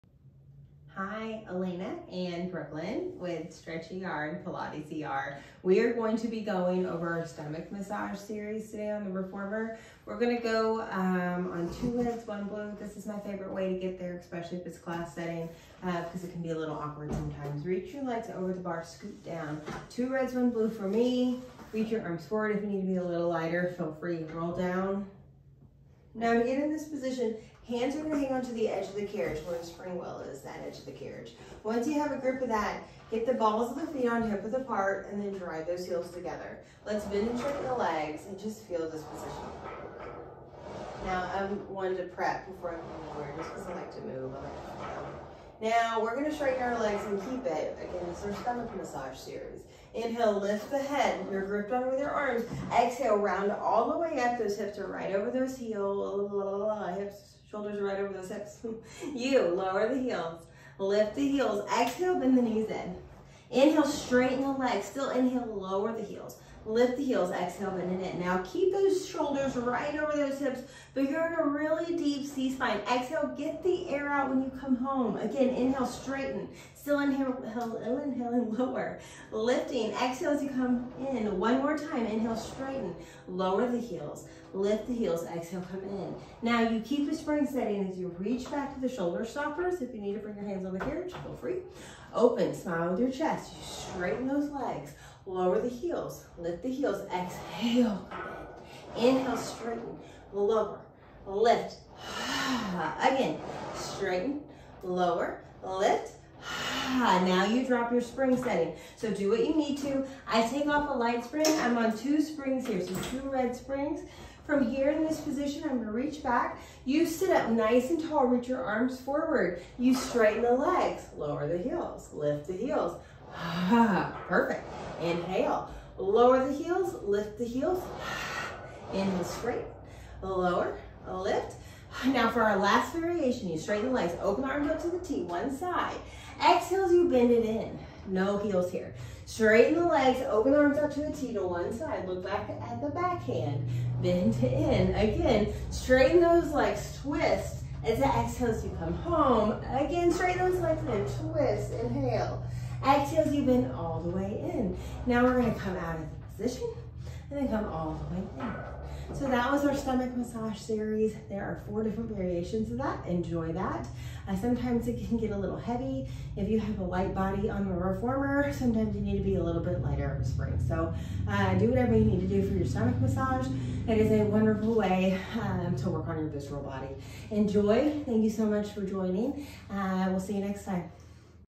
Pilates Reformer Stomach Massage Series
Pilates Reformer stomach massage series. All about the breathe, exhale all you got. Change the spring if its too hard to hold body position. Enjoy!